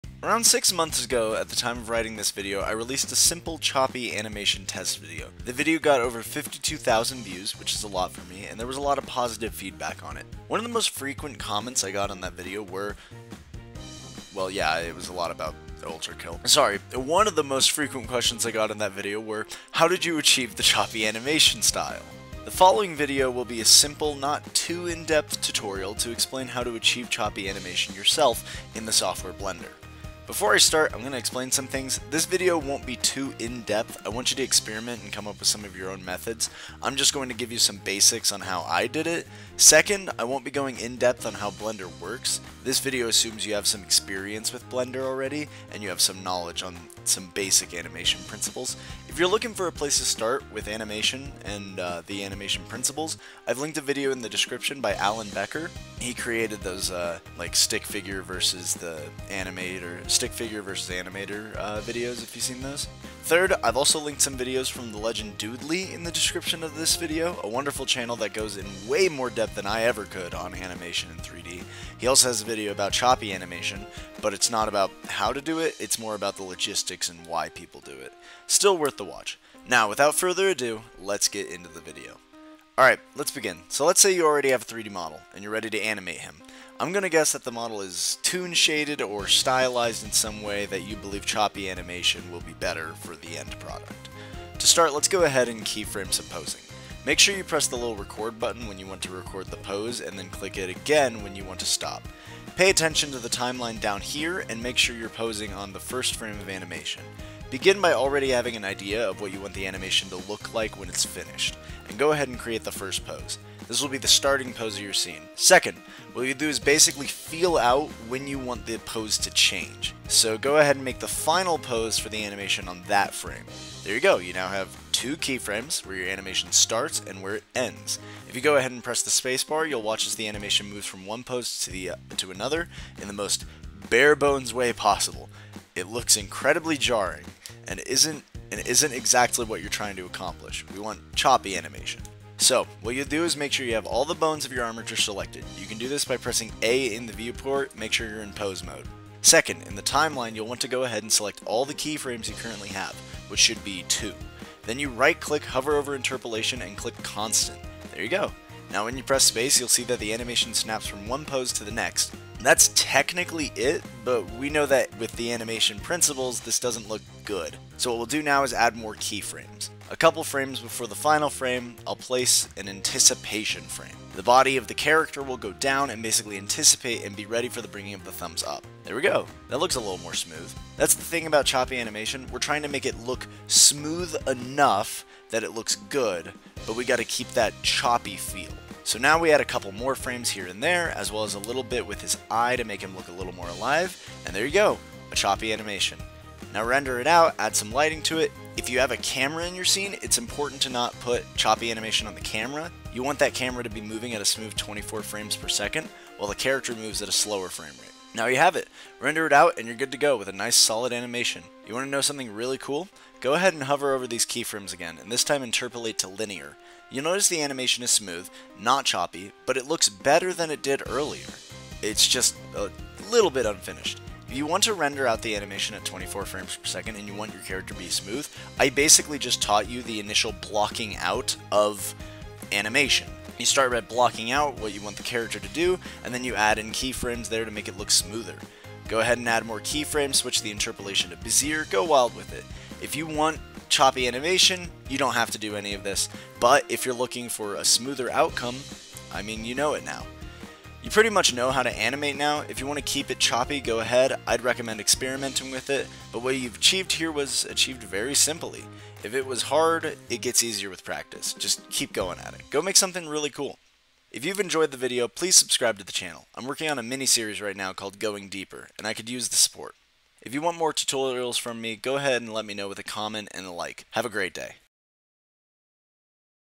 HOW TO DO CHOPPY ANIMATION IN BLENDER
This is a short tutorial to explain how I'm able to achieve Choppy animation. Despite the fact I don't use Choppy animation that much anymore it's still important to note how keyframing out and constant interpolation can still look stylized while in a choppy style.